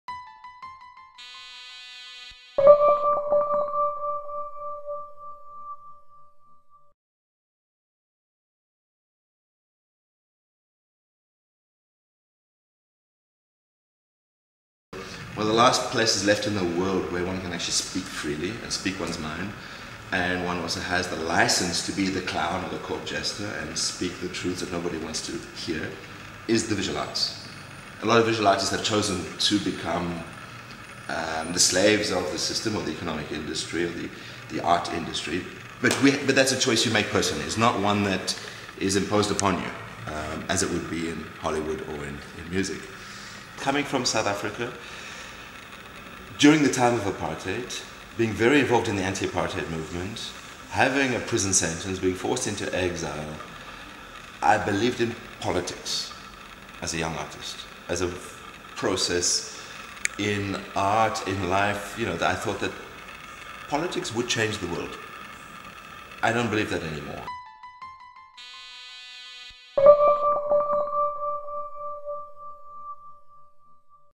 ImagineFashion.com Presents "Abandoned - Kendell Geers" by Casey Spooner & Adam Dugas
Violent, shocking, dangerous and arresting - the challenging artworks, performances and actions by the artist Kendell Geers reflect back unsettling truths of our lives today. Exiled from his native South Africa during Apartheid, Belgian-based Geers creates works that challenge our received notions about politics, language, human nature and our place in the world. "No Government, No Cry," his recent exhibition and manifesto inspired by the lack of government in Belgium (12th June marked one year) invited a group of artists to participate in coming up with solutions to the situation.